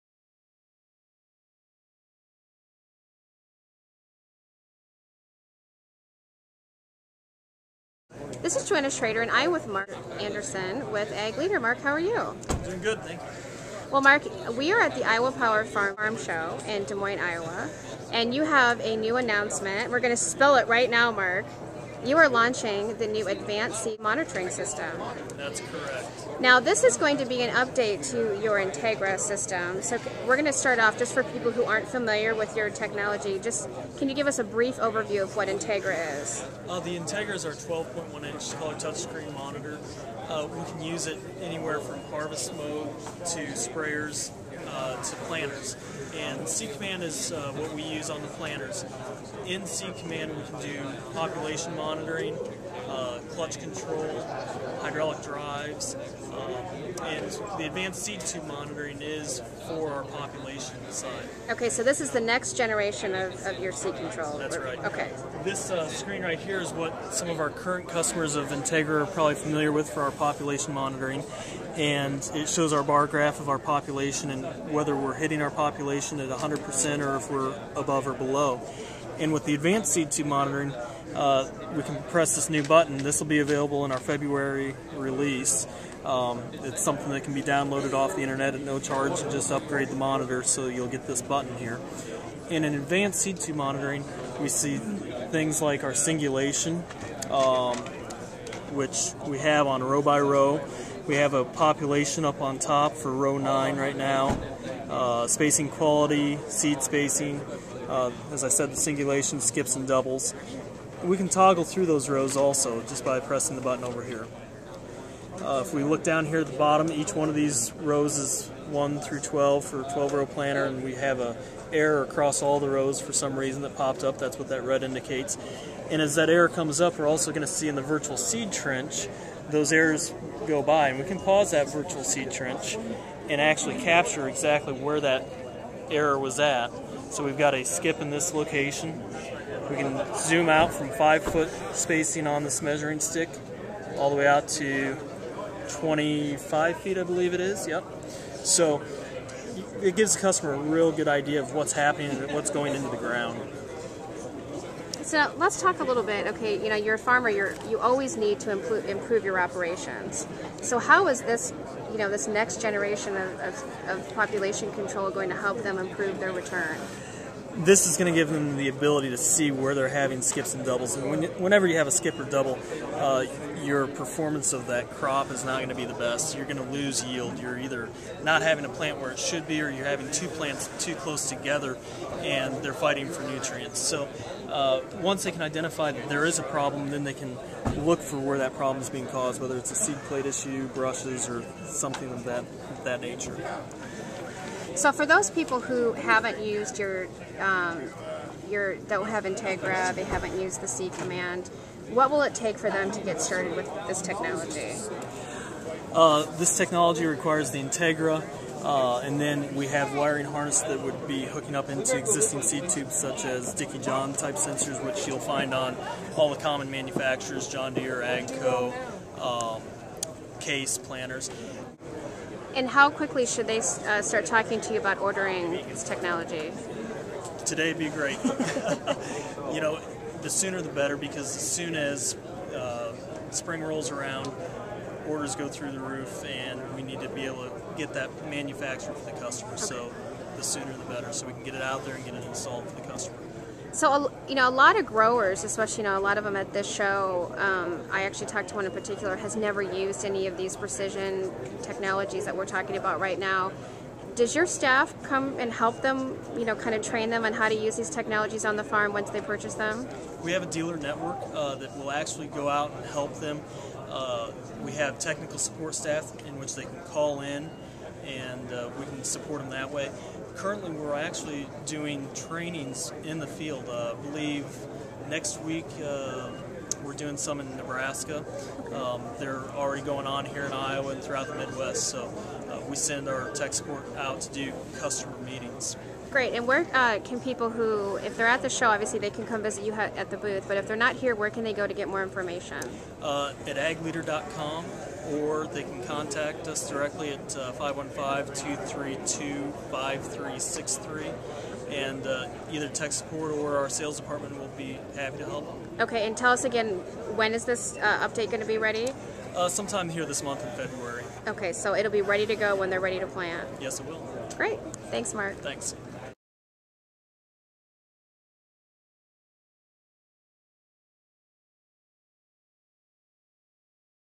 Ag Leader Advanced Seed Monitoring
Ag Leader Technology has released it's newest update to SeedCommand - Advanced Seed Monitoring. Designed to operate on their INTEGRA display, this latest precision ag technology helps growers eliminate yield robbing planter problems. Mark Anderson walks you through how Advanced Seed Monitoring works in this video.